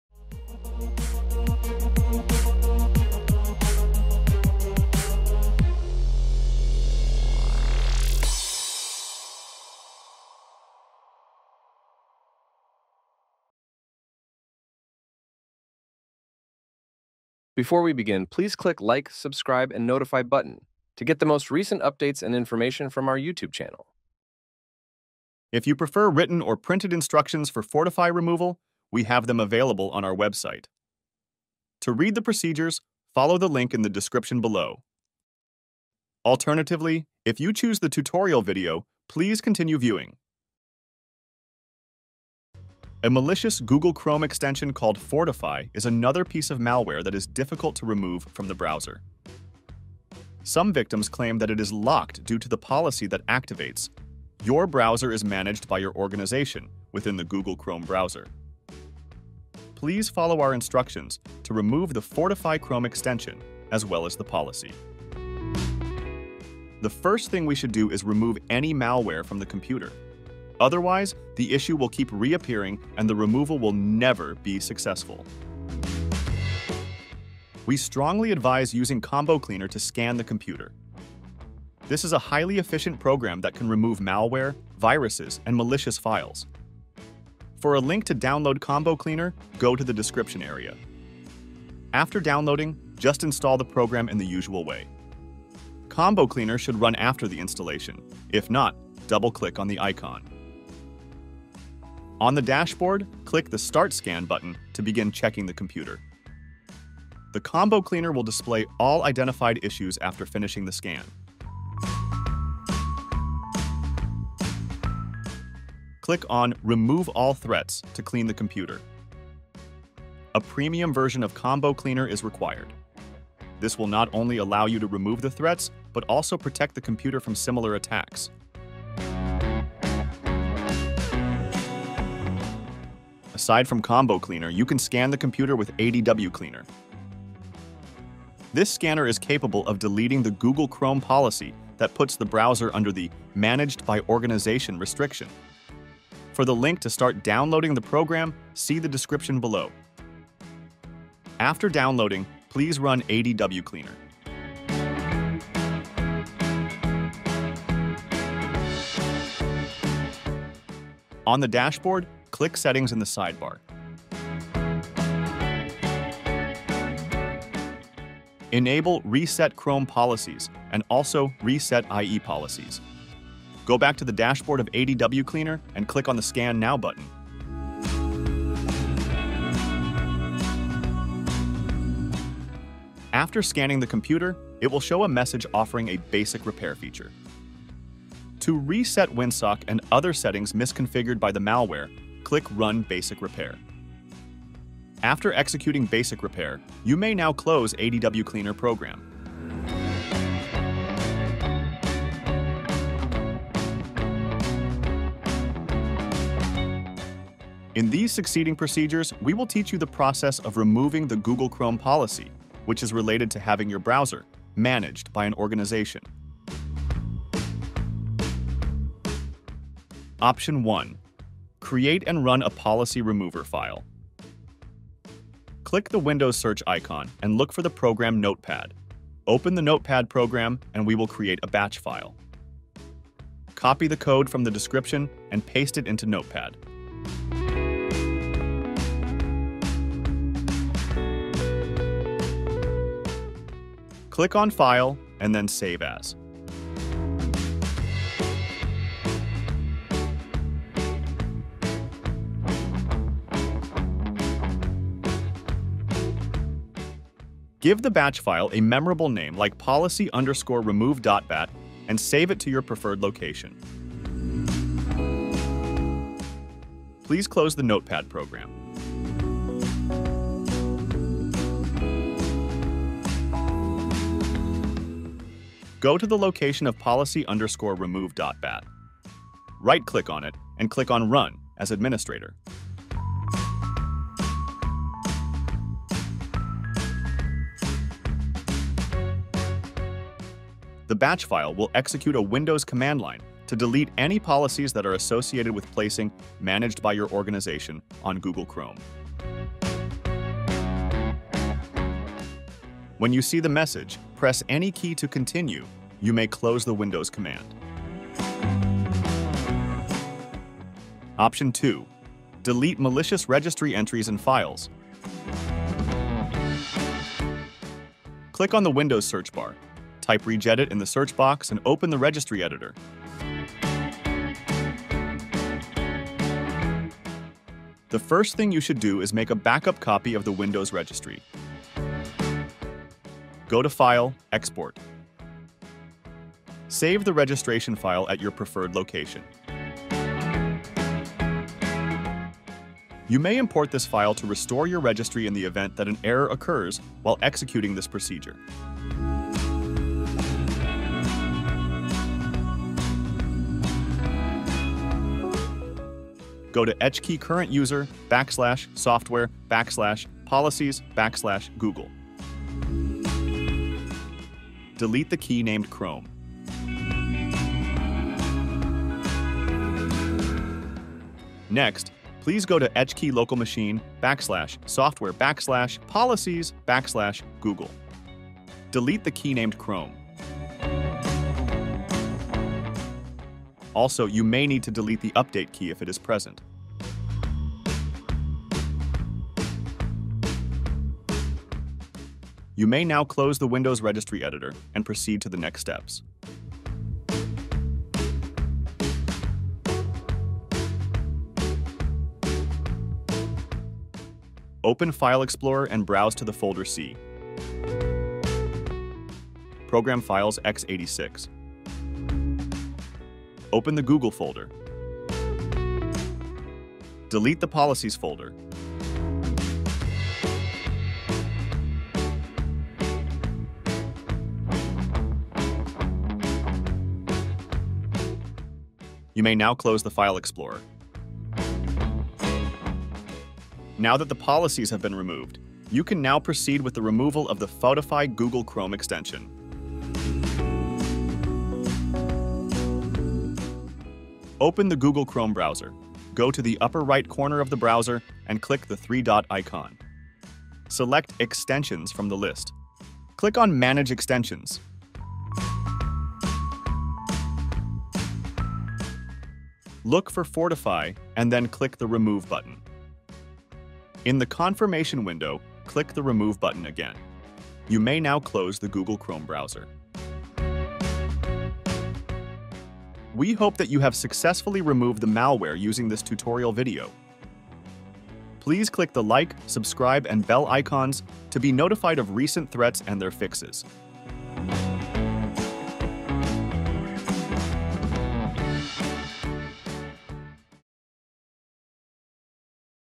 Fortify Chrome Extension Removal (Managed by your Organization)
The removal of the Fortyfy Chrome Extension is the content of this video. Also included is the procedure to remove the "Your browser is managed by your organization" policy.

How to remove FortyFy - Timeline
00:43 About Fortyfy Chrome extension.
01:09 Run Combo Cleaner scan.
02:18 Scan with AdwCleaner.
03:30 Create a policy remover file.
05:15 Delete Chrome Policies from the Windows Registry.
07:20 Remove the Fortyfy Google Chrome extension.